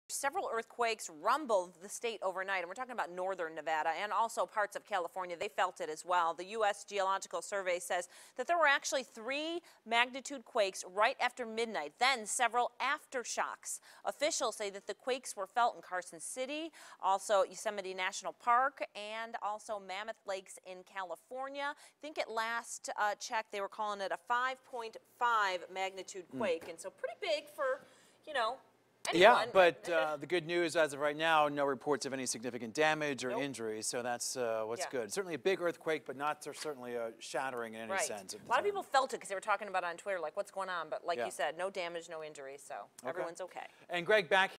5.7 magnitude earthquake strikes Hawthorne
The U.S. Geological Survey recorded three earthquakes and multiple aftershocks near Hawthorne just after midnight Wednesday.